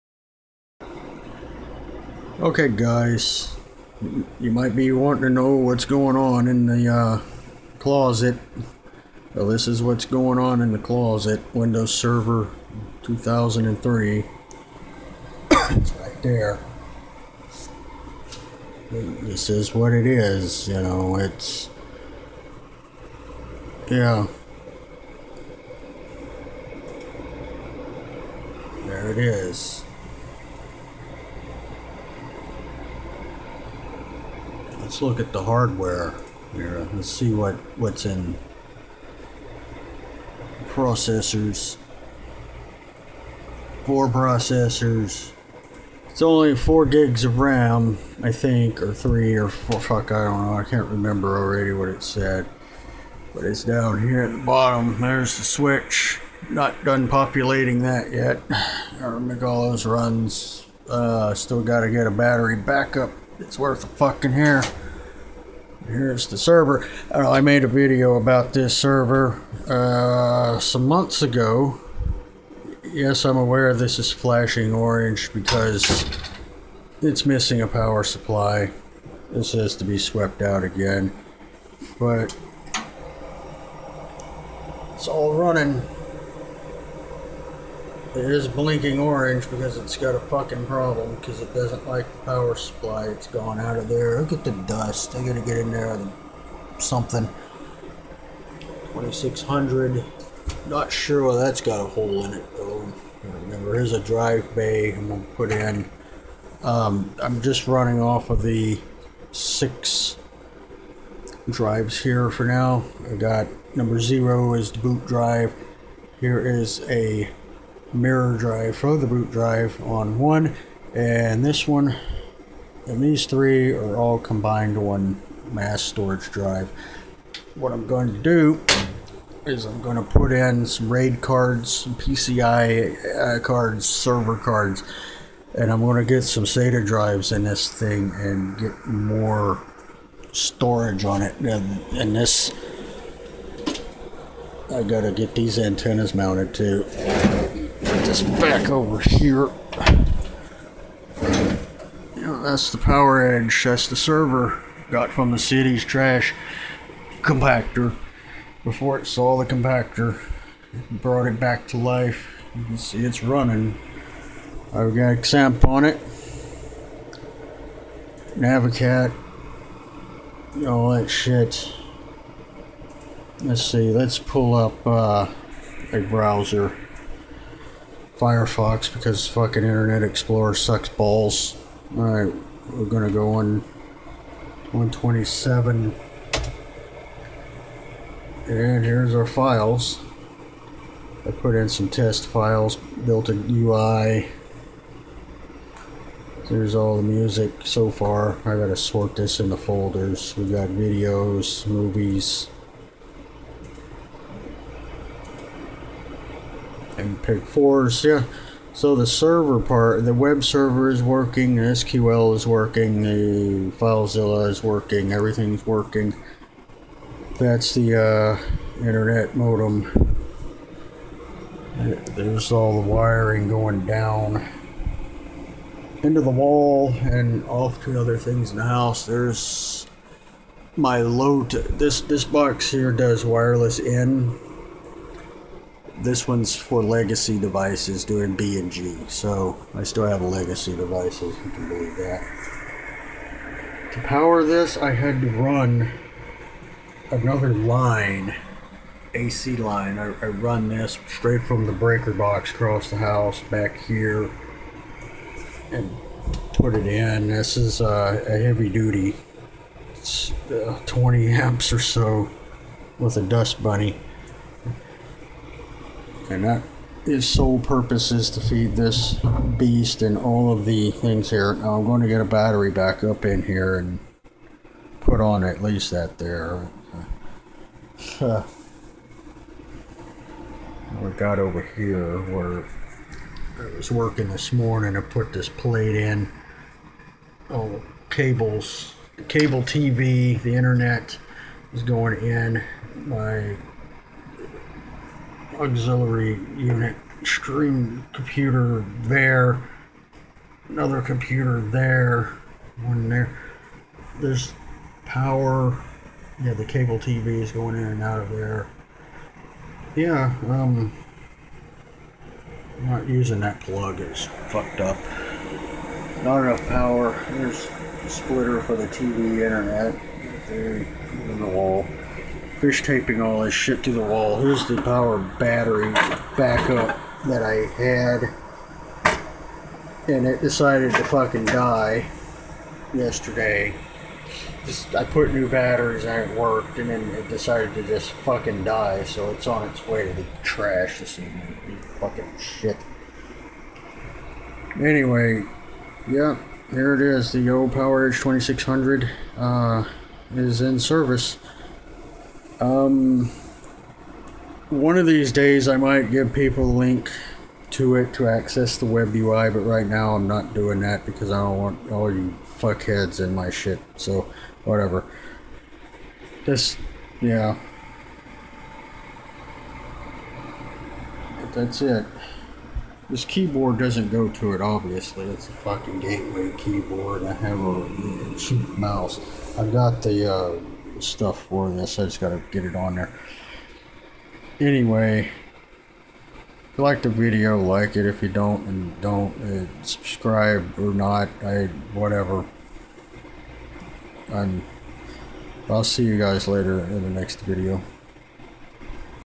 My free (restored) Dell Poweredge 2600 - NAS / Webserver
This is the same Dell PE-2600 I got from the city's IT department that was bound for the compactor. I replaced the raid battery and re-installed Windows Server 2k3 Enterprise Edition on it and set it up as a NAS with a web UI.

Yes I am aware that there is more modern and faster hardware out there but this server is much more reliable than most of the reasonable server solutions out there within my budget today. Also it works and it was free so hell yeah I'm going to use it.

Hardware Brief:

Two Intel dual core Xeon CPU @ 2.80 GHz
3 GB ECC registered  DDR  RAM
Six 68 GB SCSI hard drives
LSI Perc 4 / di Raid system
Drive #0 - OS Boot and local apps
Drive #1- Mirrors #0
Drives #2 thru #5 - Combined to create mass storage

Yes I know it's missing a power supply, thats why the led is flashing orange.

I Plan on installing a couple more SATA Raid PCI cards and loading them with 1Tb drives. I will have to engineer some drive cages for them.